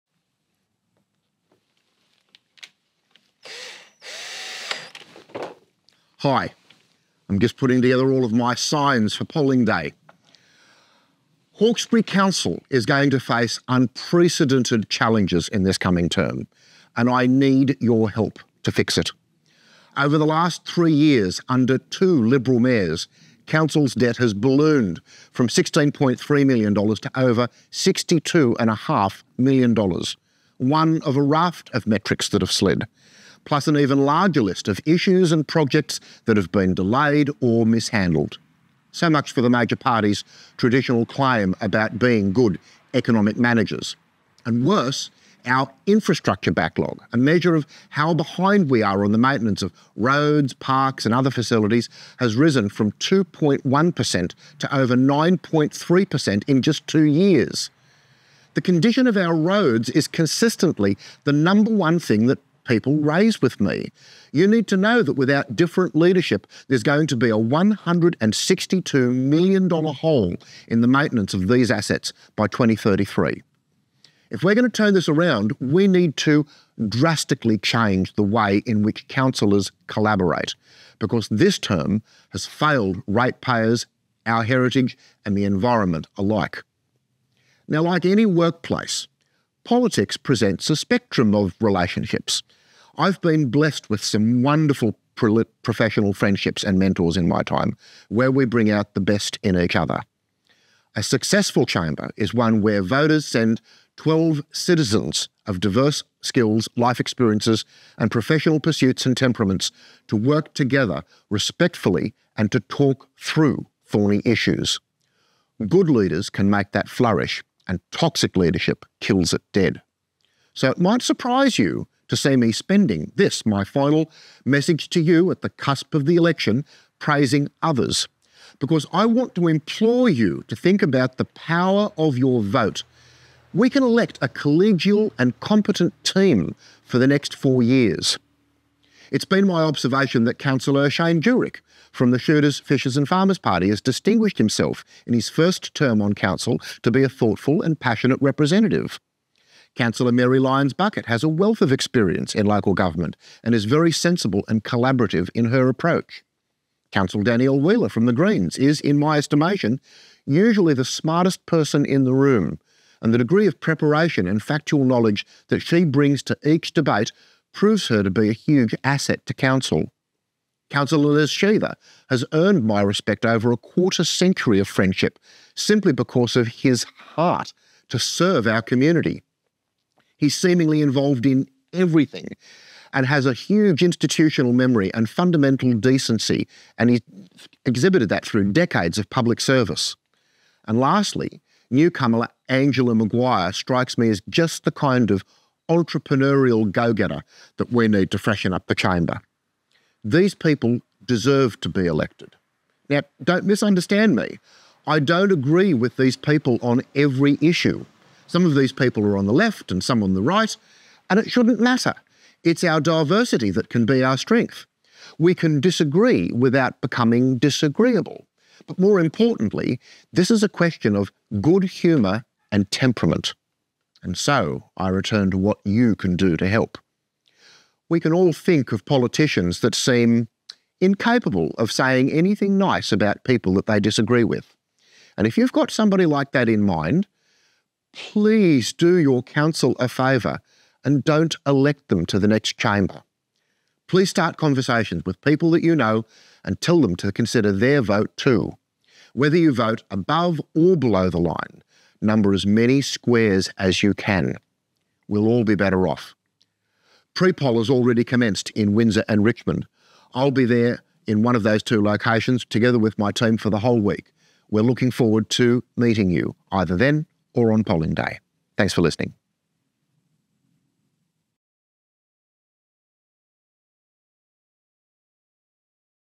Who to elect to Hawkesbury Council at this election
In this, my final video message to you before the election, I bring you what I wrote for my editorial appearing in this month's edition of the Hawkesbury Post.

It may surprise you to see me spending my time praising others. But I don't just want to get re-elected. I'm upbeat about the possibilities of, (and this is a radical idea I know), electing a team of twelve who actually like each other and can disagree without becoming disagreeable.

Please, start conversations with people you know about the importance of numbering all the squares above or below the line. You can start by sharing this post.

I and my team are in GROUP 'H' on your ballot paper.